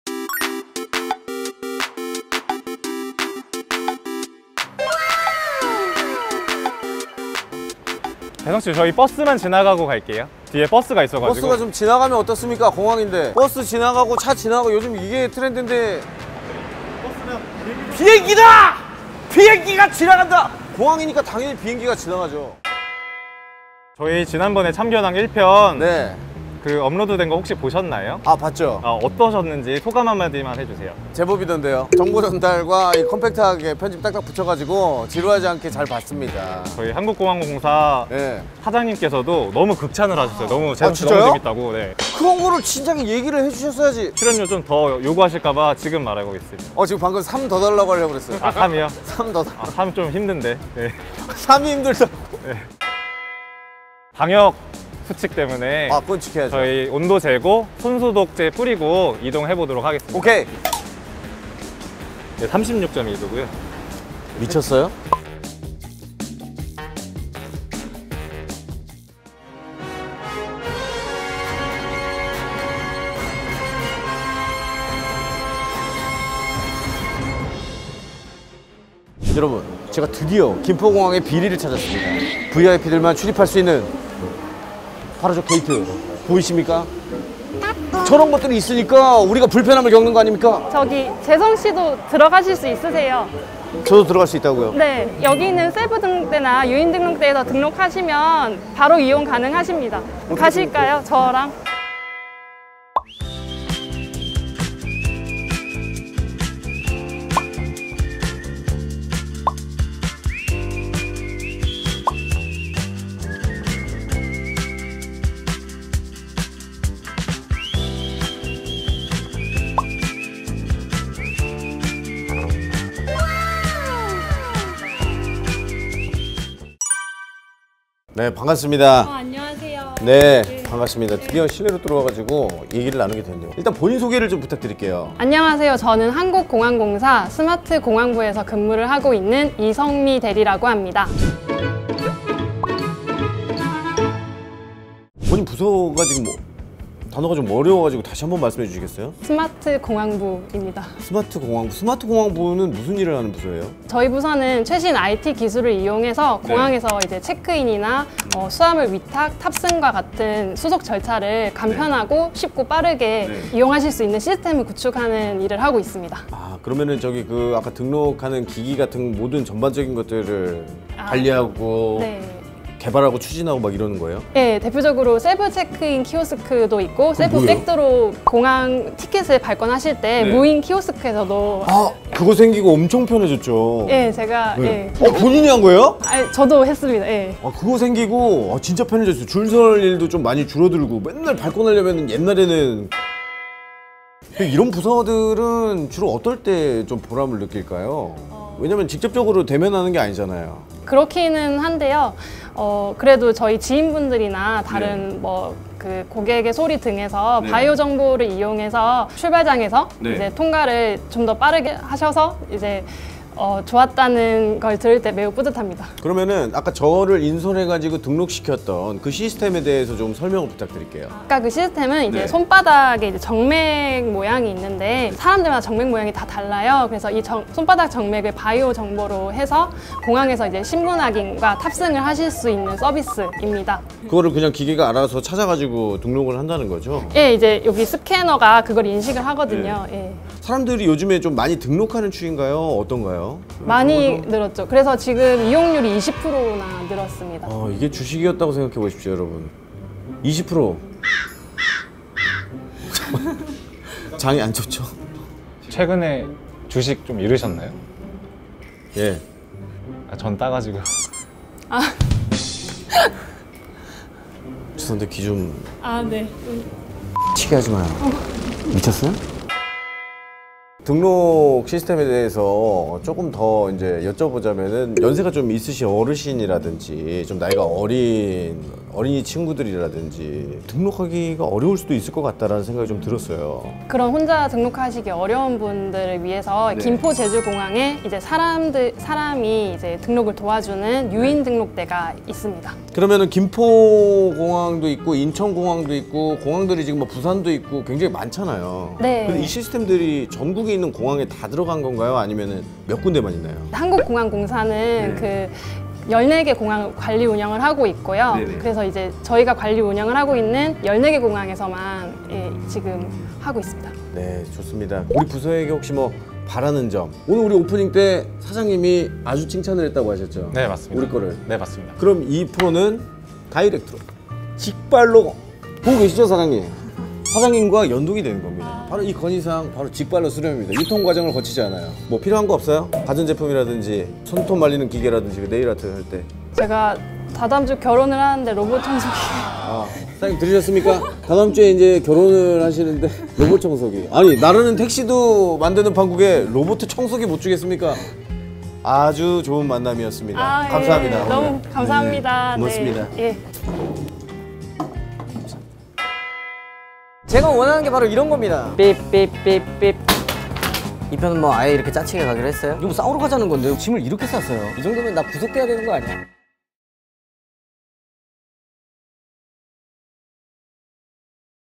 코미디의 황제(?) 황제성이 김포공항에! "등록고객게이트? 특별대우는 아니겠지?!" 참견황 Ep. 03ㅣ김포공항ㅣ바이오정보 신분확인
한국공항공사의 윗선(?)의 재부름을 받아
김포공항이 잘하고 있는지 방문한
코미디의 황제(?) 황제성.

“김포공항, 일 똑바로 하고 있어?!”

★EVENT★
“바이오정보 신분확인”에 대해 어떻게 생각하시나요?!
여러분의 생각을 댓글로 남겨주세요!
추첨을 통해 경품을 드립니다. (◞♥ꈍ∇ꈍ)◞♥

▶참여기간 : 11월 16일(화) ~ 11월 21일(일)까지

▶당첨자 선물 :
🎁별다방 아메리카노 (10명)

▶당첨자 발표 : 11월 25일(목) 영상 댓글에서 확인!